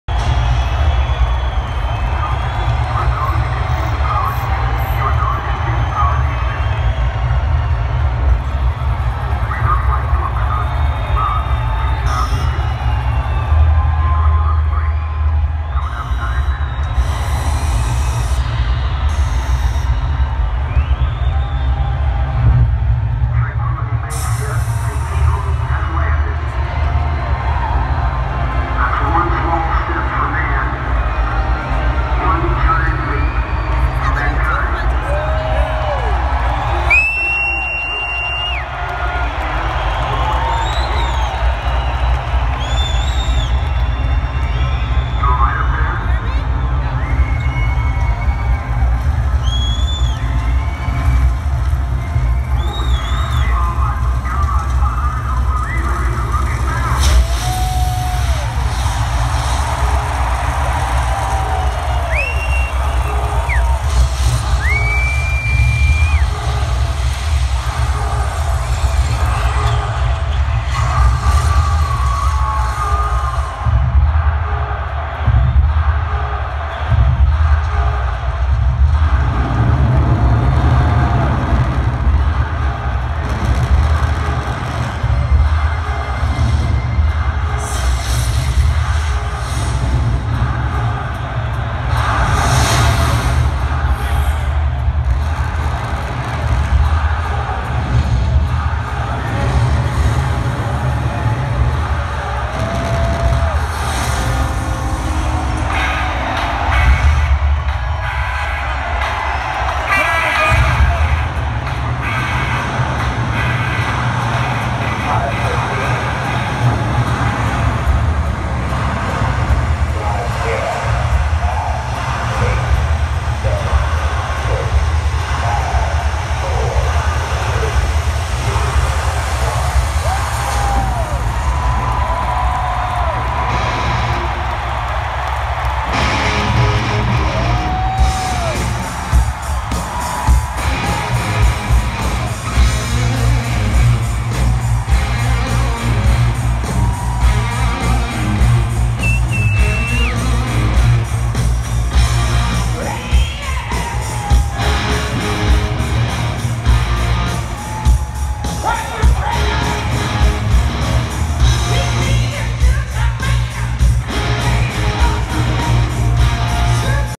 AC/DC
Scott Trade Center, February 20th 2016 St. Louis Missouri